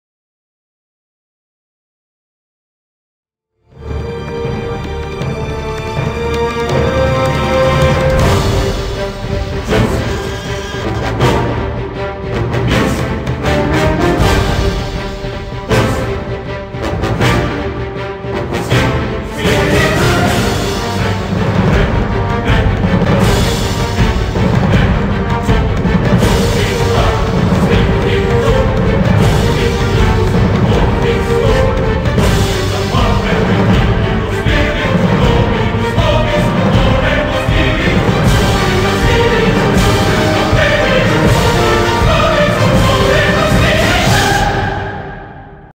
The Toy War
This was an assessment I had in a 3D class. It was supposed to be a  moving object, I expanded the idea each time i finished animating a character.